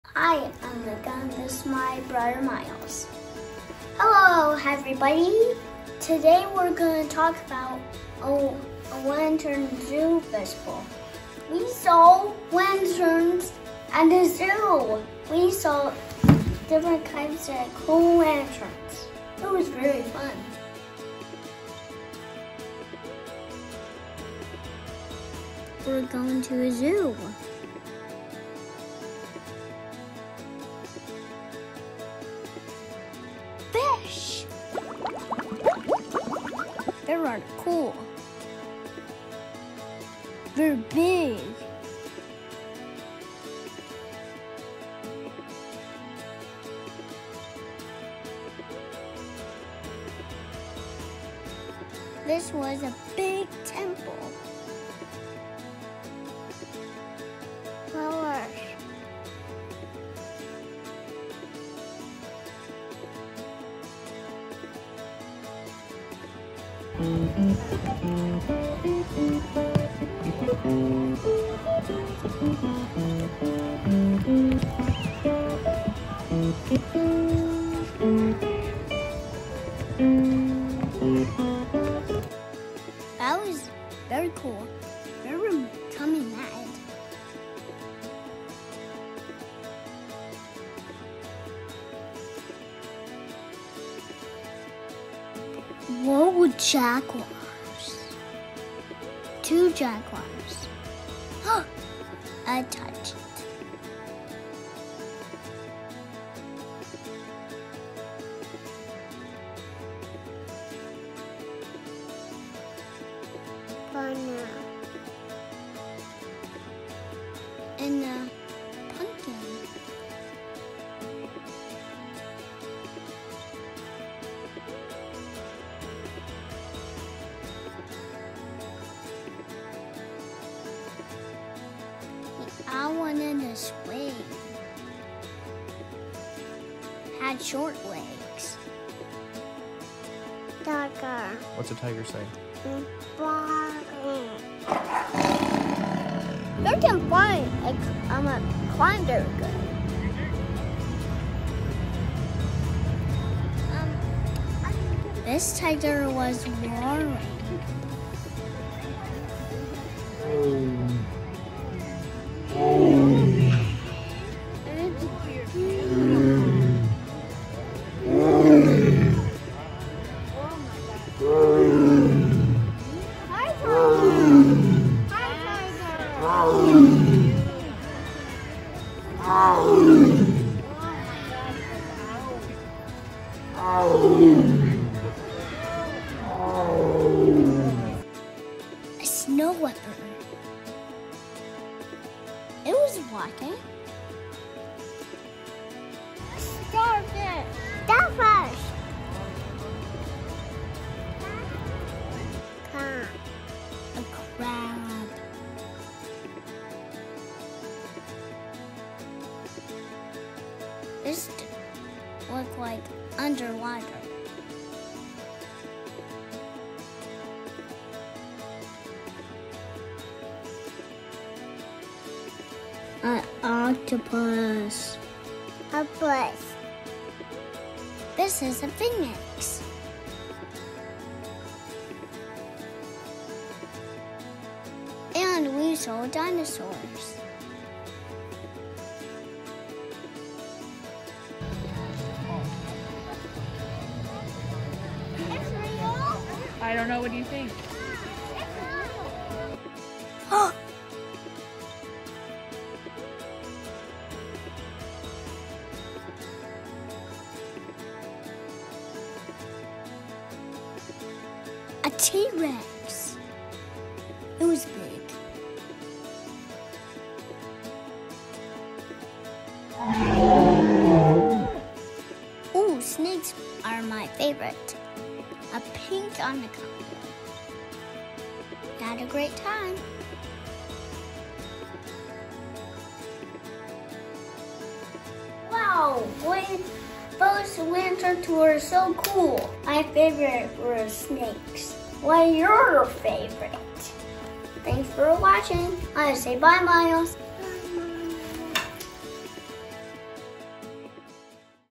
Episode 6: Asian Lantern Festival | What You Know About Animals?
This week we show you the Asian Lantern Festival at the Cleveland Metroparks Zoo! We had a great time and saw some amazing lanterns!